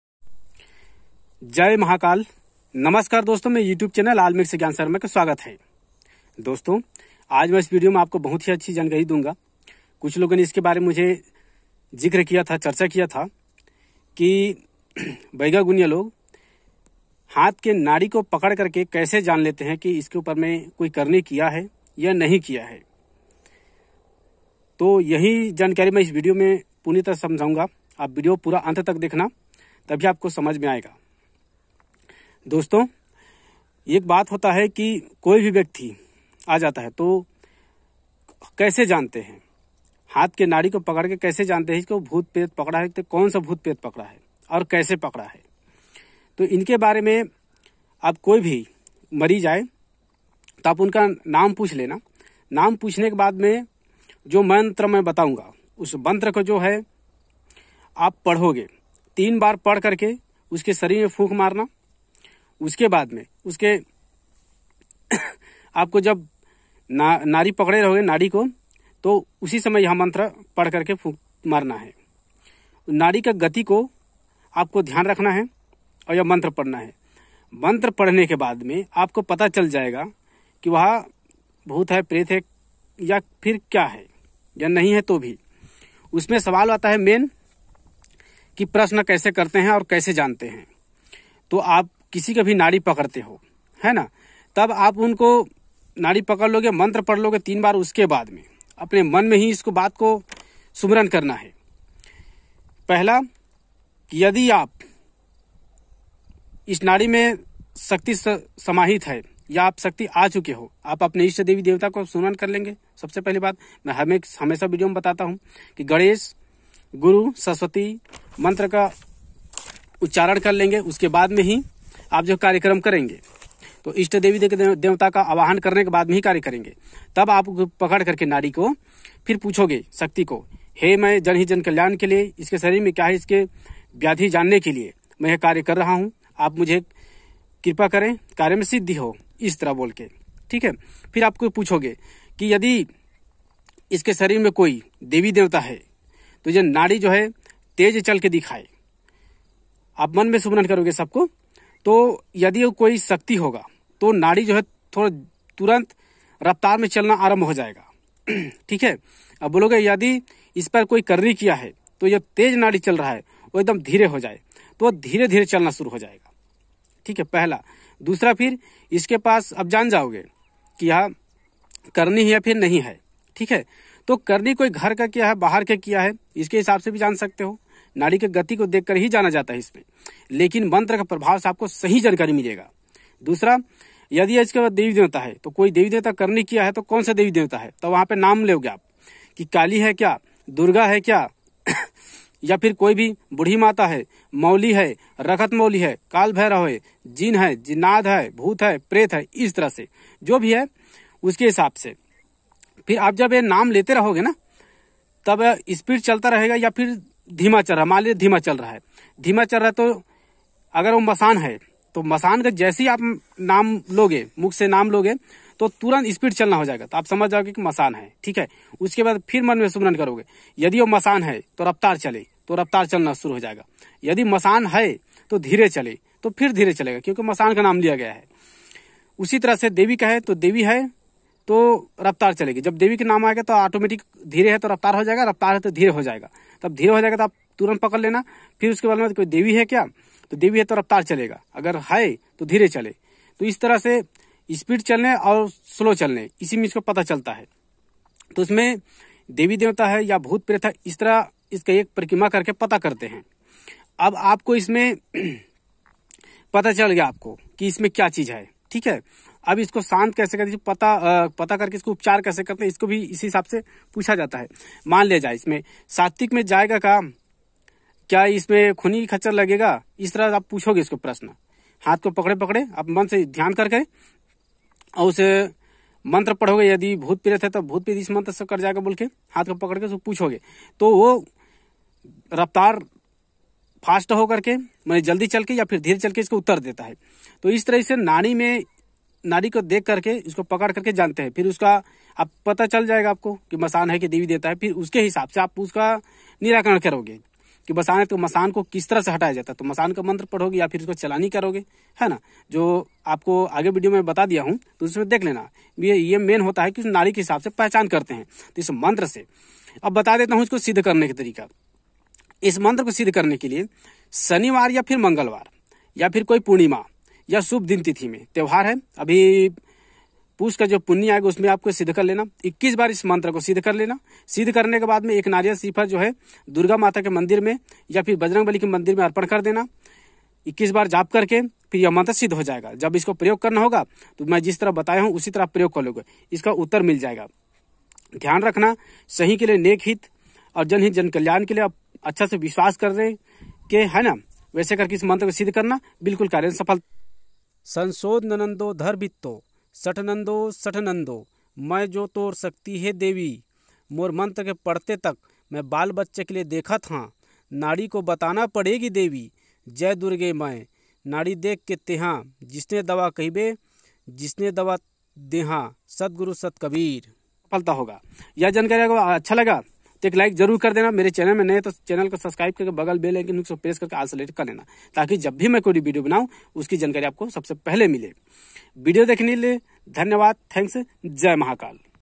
तांत्रिक यही मंत्र से //नाड़ी छुकर //जाने सभी बाते//
नमस्कार दोस्तों
इस वीडियो में कोई भी तांत्रिक किस तरह नाड़ी छुकर के मंत्र से कैसे जानते हैं कोई भी देवी देवता भूत प्रेत किया कराया उसी के बारे में वीडियो में जानकारी दिया गया है वीडियो देखने के लिए धन्यवाद।।
मेरे अन्य विडिओ...